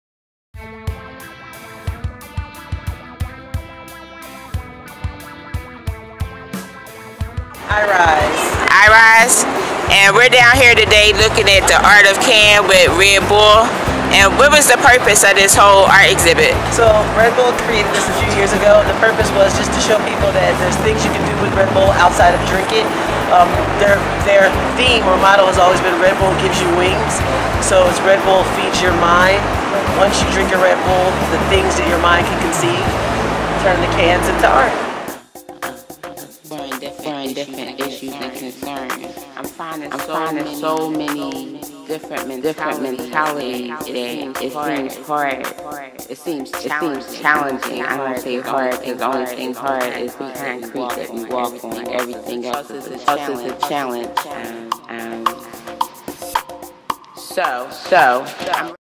Red Bull Art of Cans at Dilworth Park
RED BULL ART OF CAN IS A NATIONWIDE HUNT FOR CREATIVITY.

Red Bull Art of Can is a nationwide creative competition. We invite designers, animators, sculptors, and artists of all disciplines to create 3D art, physical or digital, using the Red Bull can as inspiration.
This year, more than 100 artists registered work for the exhibition. A selection was made by a panel of judges from the art community to be displayed at a special showcase at Dilworth Park in Center City Philadelphia October 1 - 8, 2015. @redbullphilly